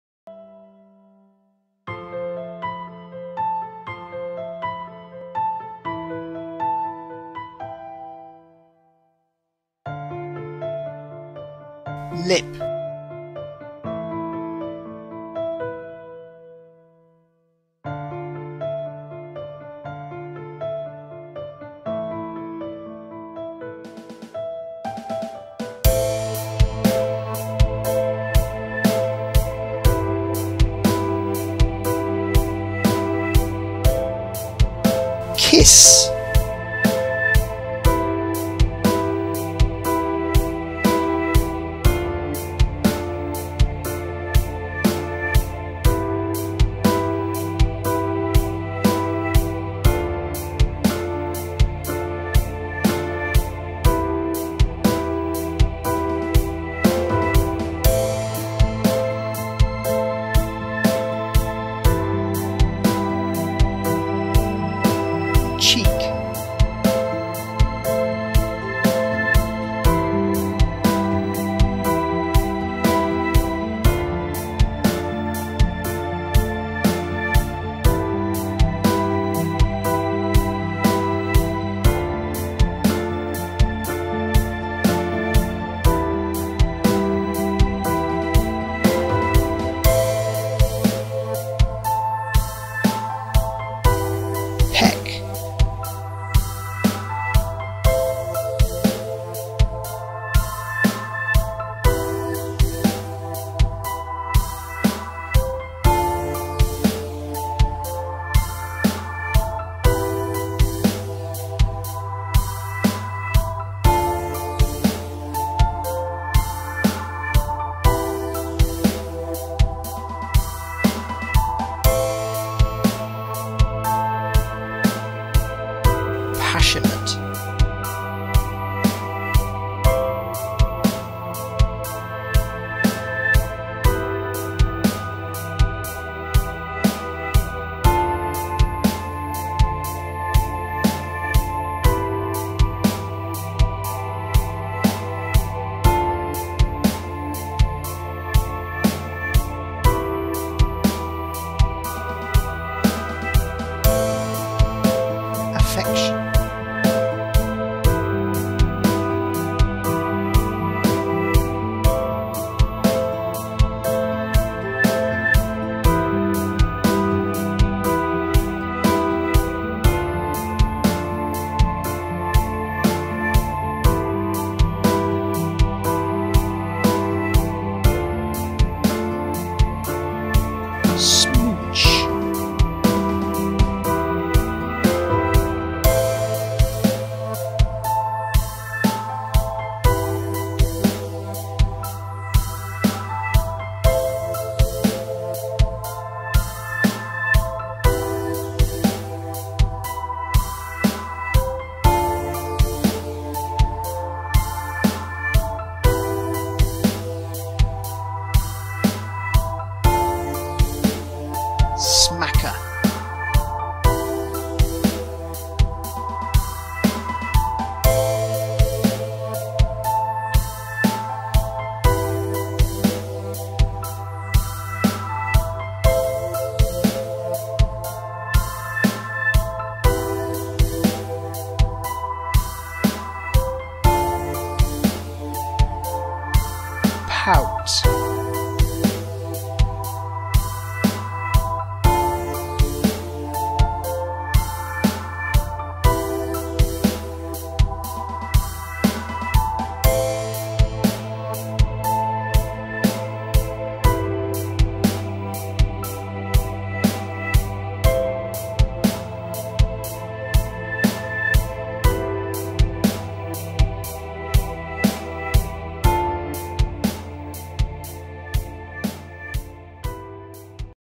Vocabulary Lesson Kissing
English vocabulary lesson with words connected to kissing. This lesson will help you define lip, kiss, cheek, peck, passionate, affection, smooch, smacker and pout.

(A) lip NOUN one of the two soft, red parts of the mouth
‘Their lips were about to meet for the first time.’

kiss VERB to touch someone with your lips as a sign of love or greeting
‘They kissed for the first time – wow!’

(A) cheek NOUN the soft part of your face below your eyes
‘She put her hands on his cheeks and then kissed him.’

(A) peck NOUN a quick light kiss on your cheek
‘She gave him a quick peck on the check then flew away.’

passionate ADJECTIVE showing very strong feelings or emotions
‘The way he held and kissed her showed that he was a very passionate man.’

affectionate ADJECTIVE showing that you like or love someone or something
‘He gave her an affectionate kiss.’

smooch VERB to kiss, hold, and touch someone in a sexual way
‘Five minutes after they met they were smooching.’

(A) smacker NOUN a loud kiss
‘The smacker he gave her was heard by everyone in the bar.’

pout  VERB to push out your lips to look more sexually attractive
‘Her pouting was not noticed so she gave up.’